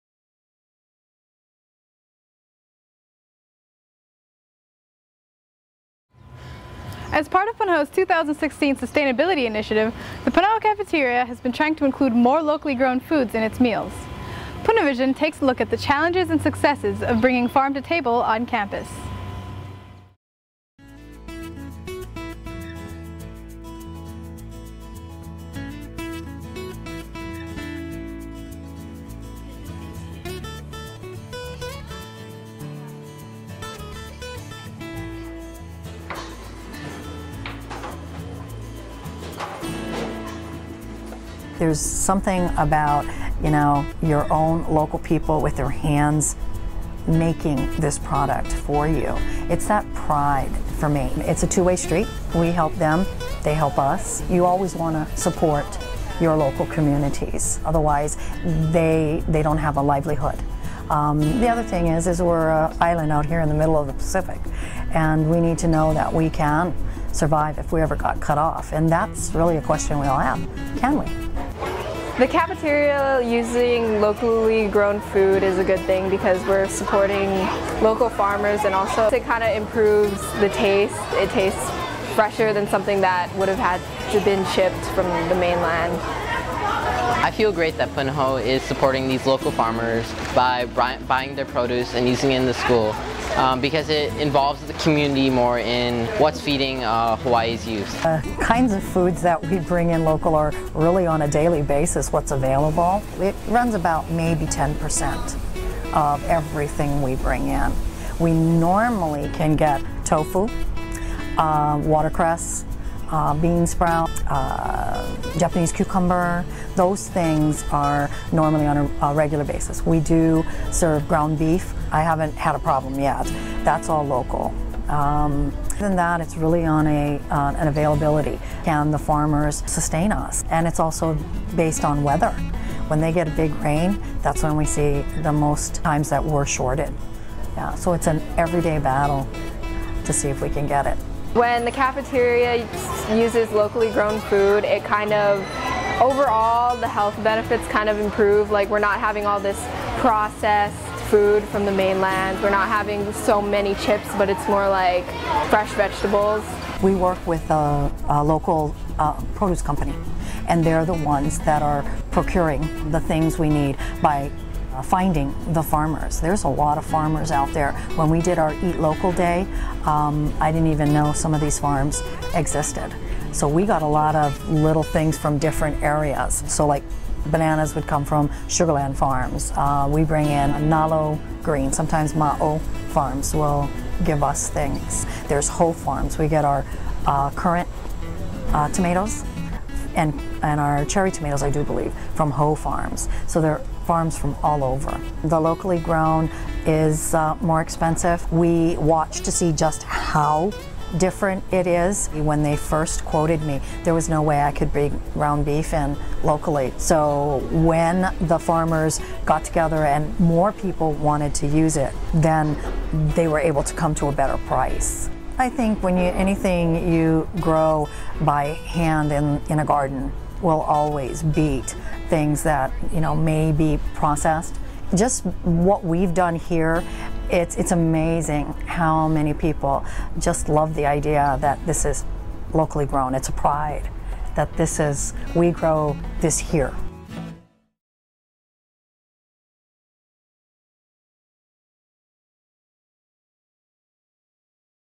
Local Foods at Punahou (Punavision - June 2012)
As part of Punahou's 2016 Sustainability Initiative, the Punahou cafeteria has been trying to include more locally-grown foods in its meals.  Punavision takes a look at the challenges and successes of bringing farm to table on campus.

Produced & edited by Kimi Mesick '14